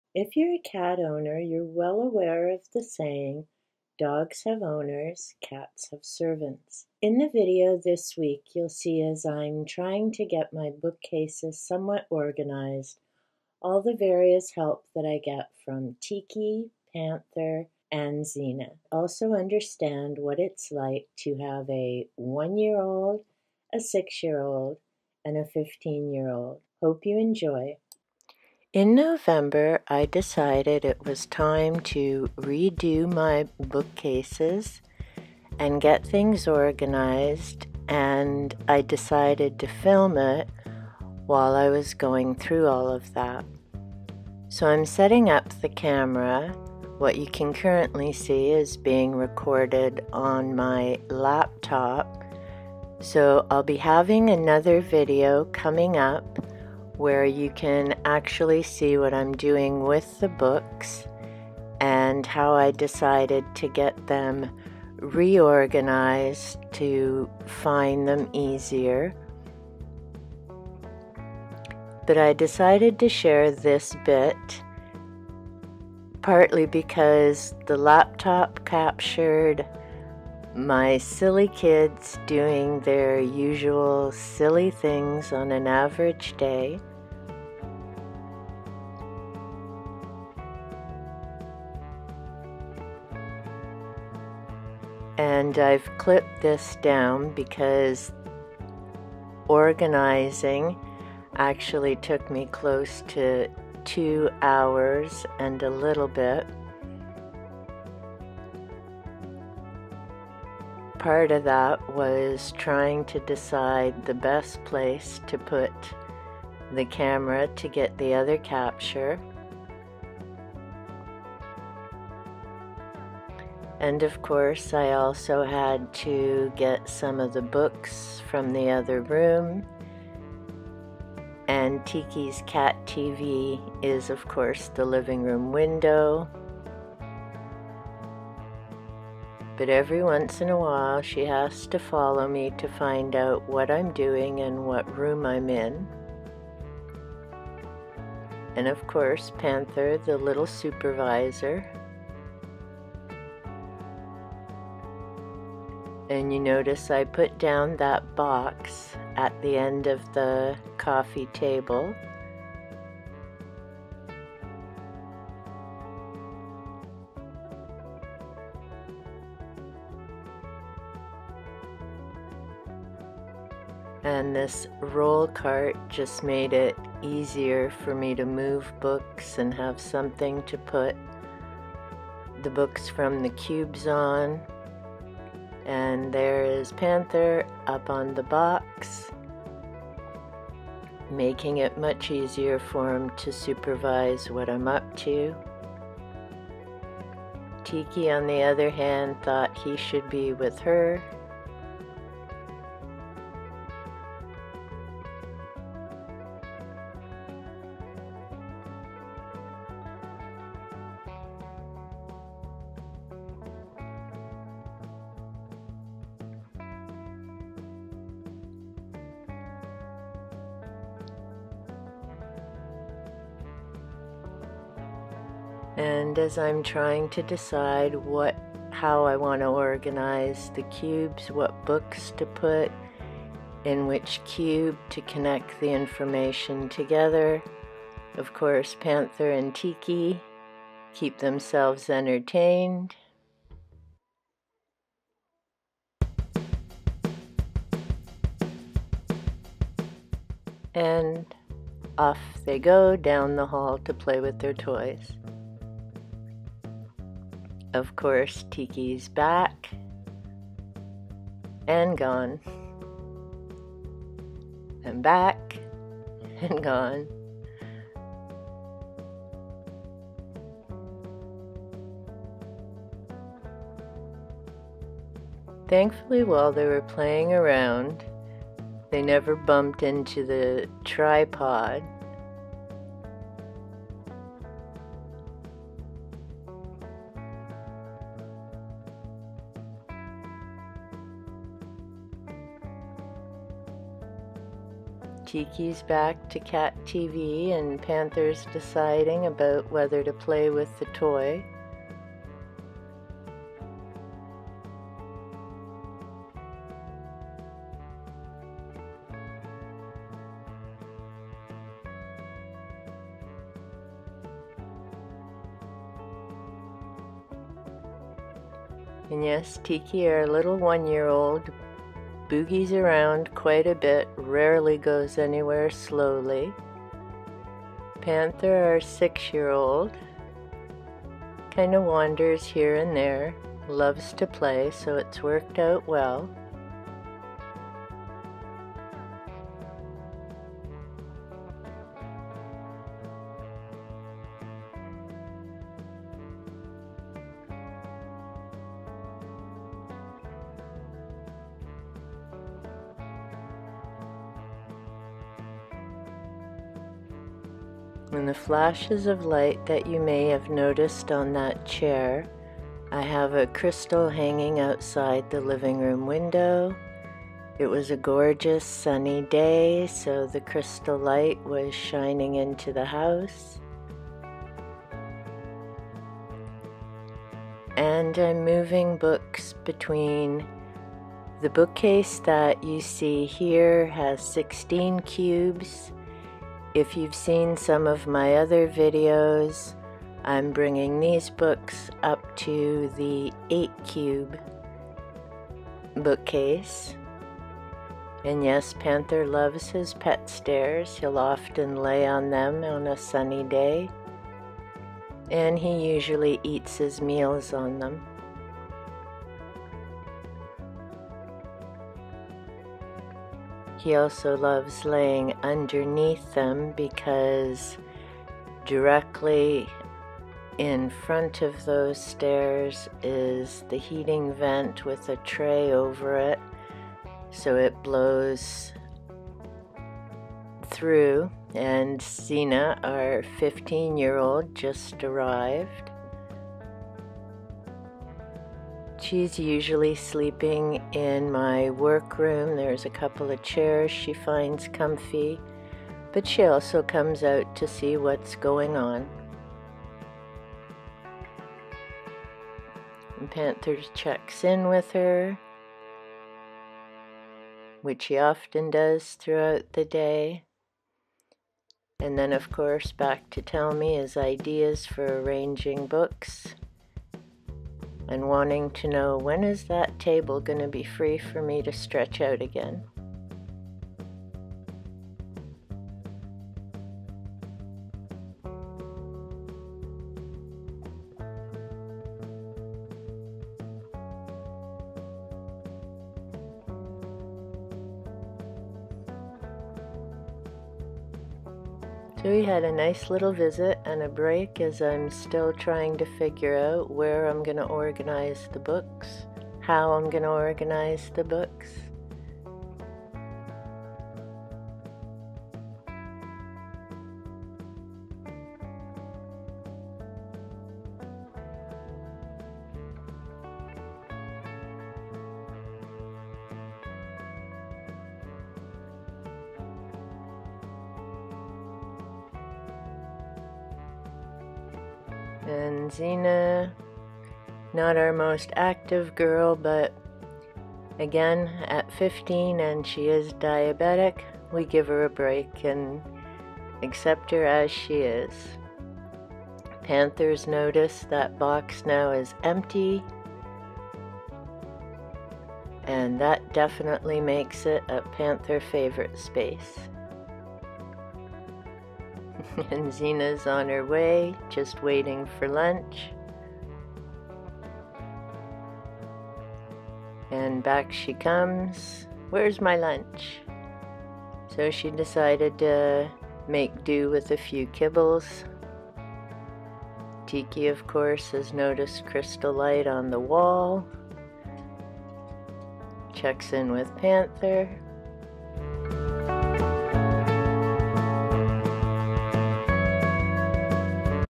TGIF - Cats and book case organizing Ep 6
Dogs have owners, cats have servants.  If you are a cat owner you will appreciate how true that is.  Tiki and Panther offer some help and of course get distracted by play time.  Zena on the other hand just wanted to know when lunch was going to be ready.